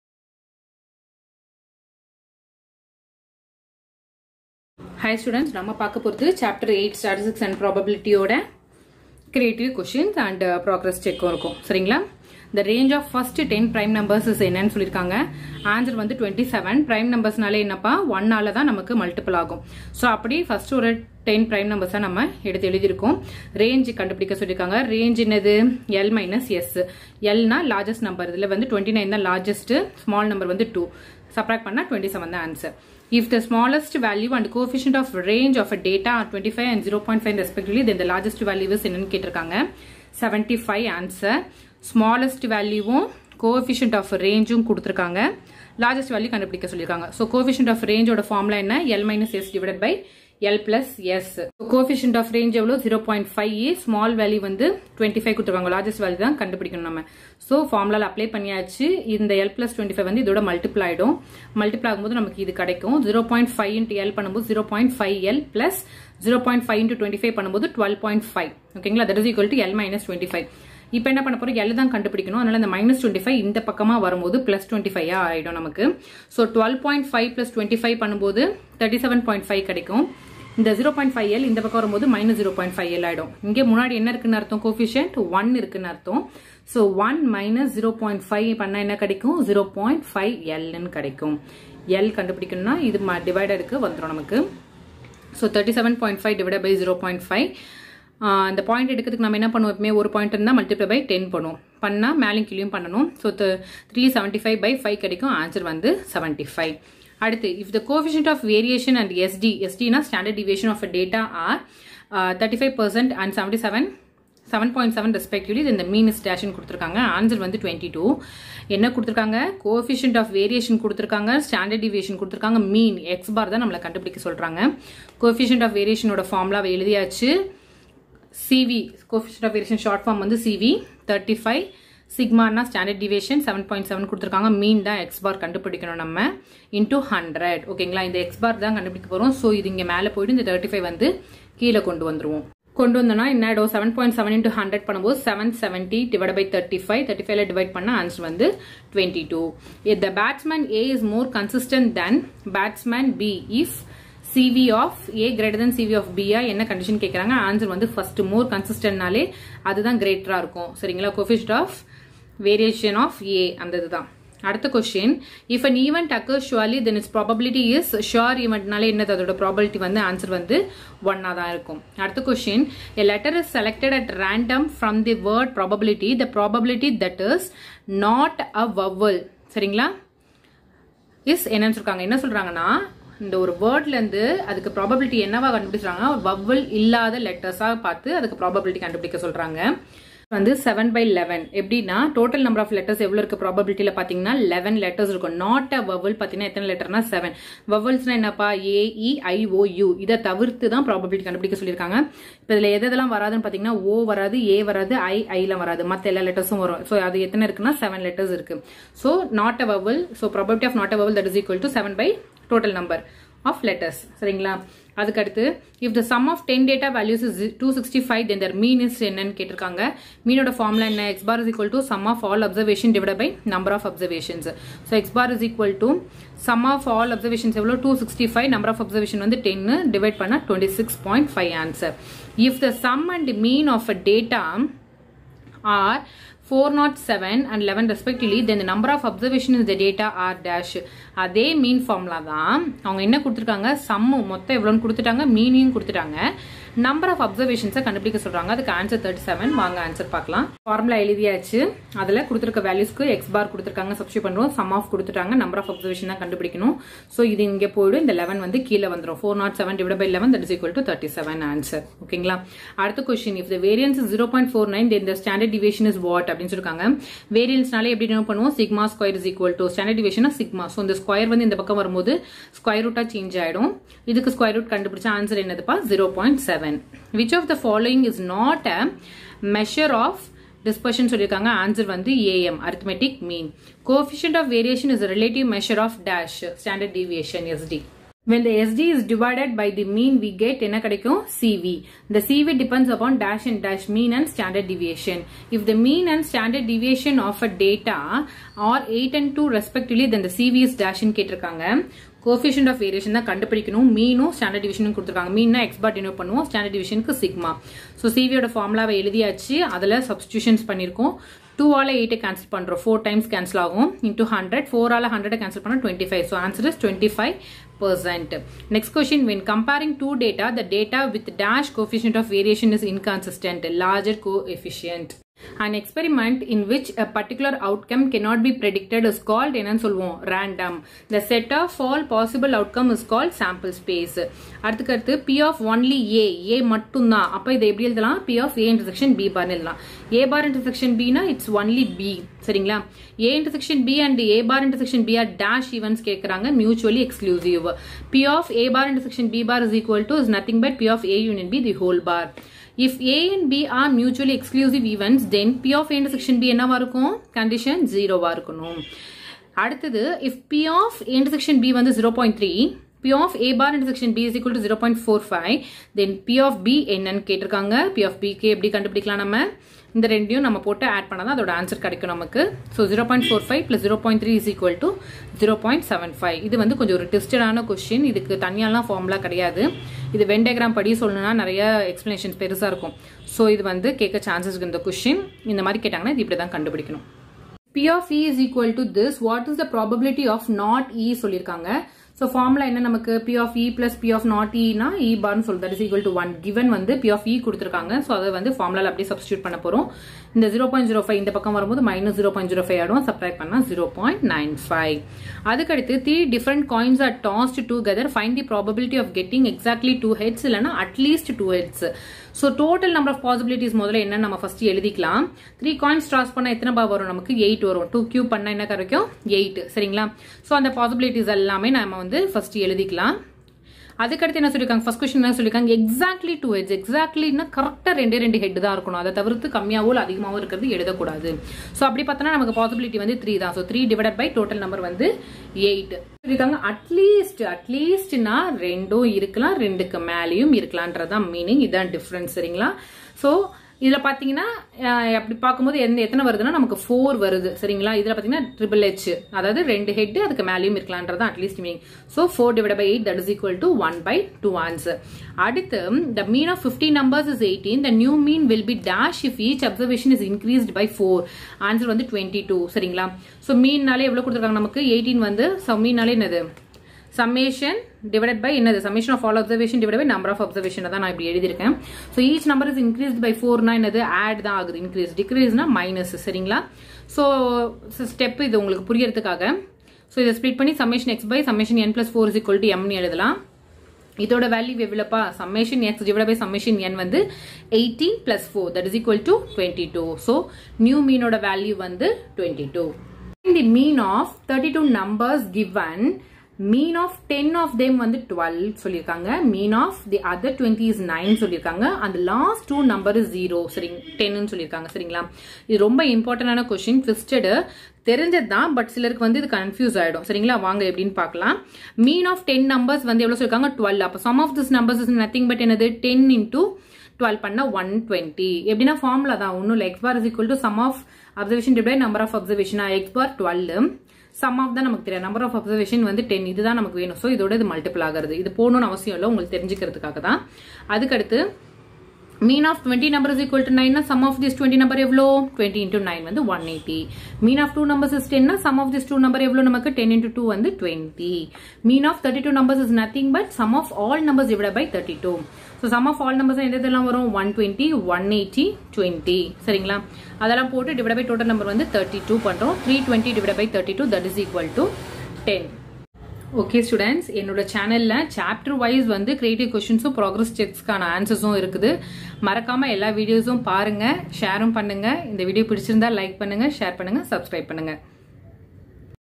10th maths (2019-2020) chapter 8 creative questions |SAI GURU MATHS
In this video we are going to learn 10thmaths new syllabus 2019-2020 chapter 8 creative and progress check questions with answers.
It's very useful to all 10th students